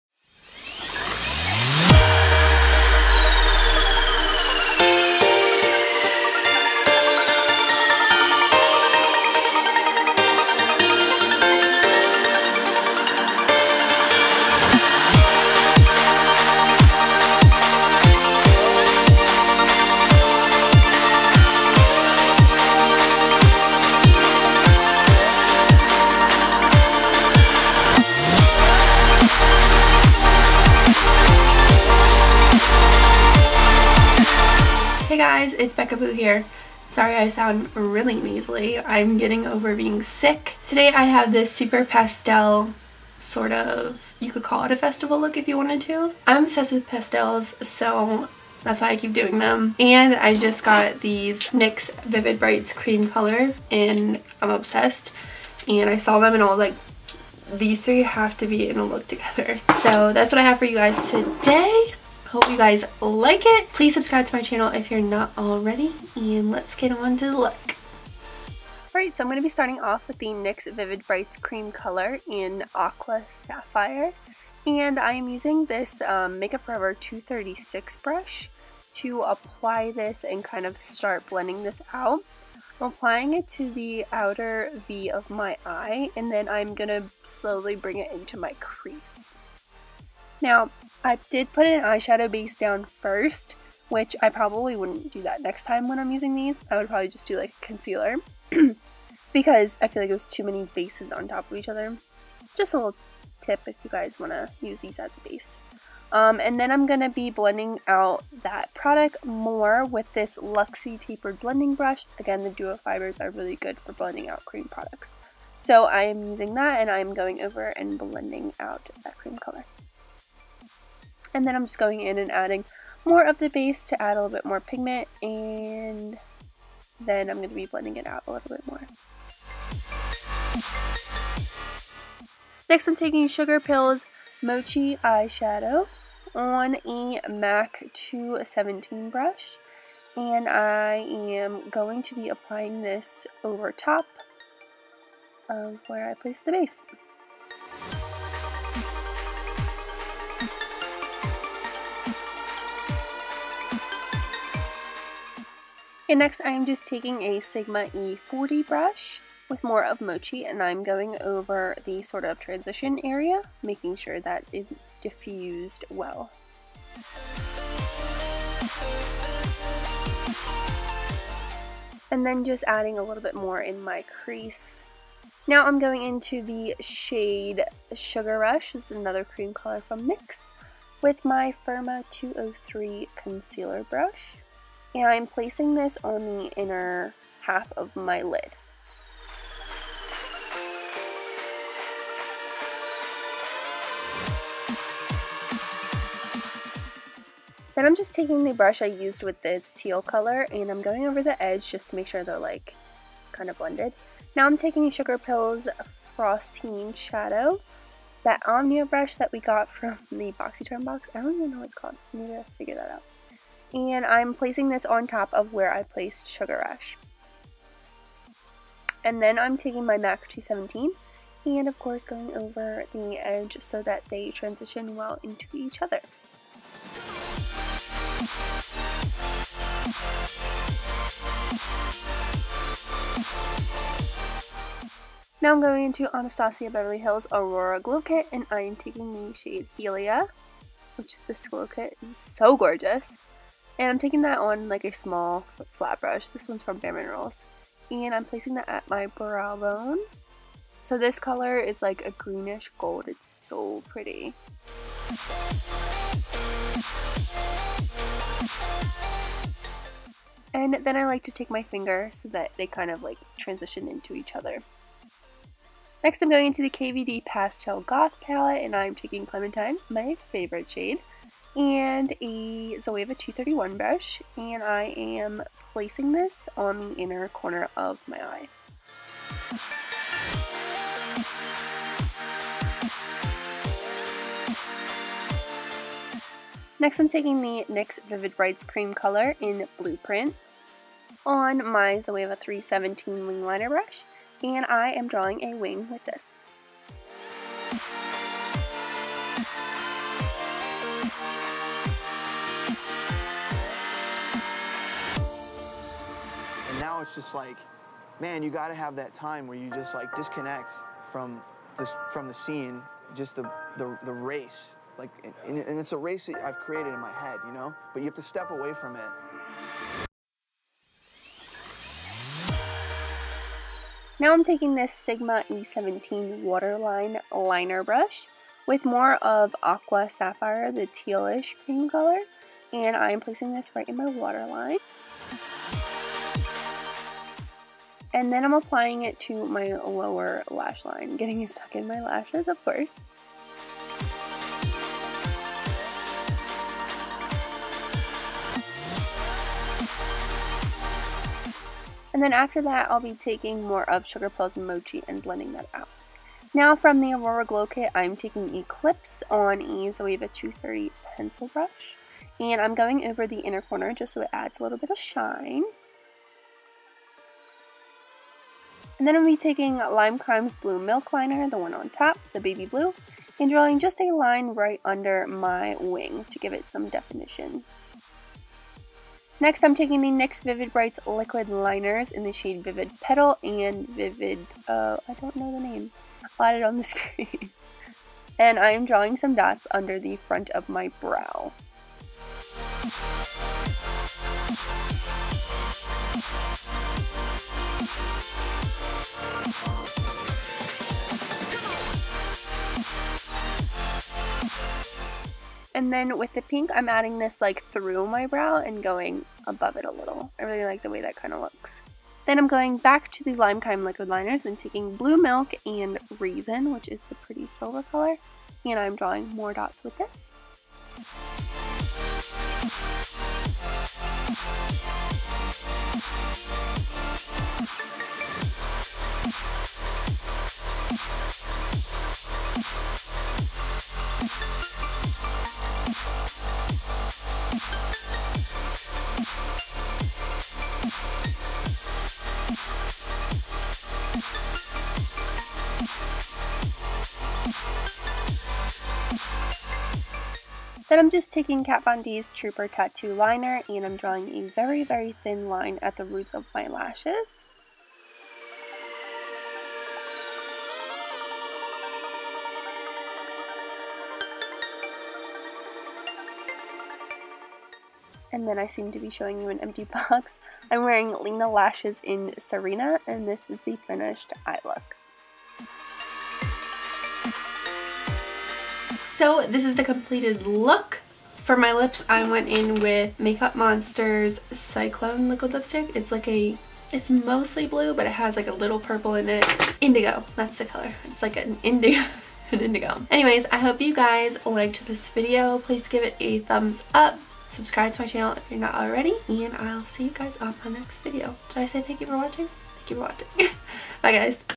PASTEL LOOK with DOTS ft NYX VIVID BRIGHTS CREME COLOURS | beccaboo
I'm hooked on PASTELS!!! ahhhhhh!!! Was so excited to use the new nyx vivid brights creme colours though!!! Hope you guys like this look!

DETAILS BELOW:
*NYX Vivid Brights Creme Color- Aqua Sapphire
*MAKEUP FOR EVER 236 brush
*LUXIE Tapered Blending brush
*SUGARPILL Mochi
*NYX Vivid Brights Creme Color - Sugar Rush
*FIRMA BEAUTY 203 brush
*SUGARPILL Frostine
*Royal & Langnickel Omnia brush
*ABH Aurora Glow Kit- Helia
*Bare Minerals brush
*KVD Pastel Goth Palette- Clementine
*ZOEVA 231 brush
*NYX Vivid Brights Creme Color -Blueprint
*ZOEVA 317 brush
*SIGMA E17 brush
*ABH Aurora Glow Kit- Eclipse
*ZOEVA 230 brush
*LIMECRIME Blue Milk Liquid liner
*NYX Vivid Brights Liquid Liners- Vivid Petal & Vivid Blossom
*LIMECRIME Blue Milk & Reason Liquid liners
*KVD Trooper Tattoo Liner
*LENA LASHES "Serena" Lashes

Wearing MAKEUP MONSTERS Cyclone Liquid Lipstick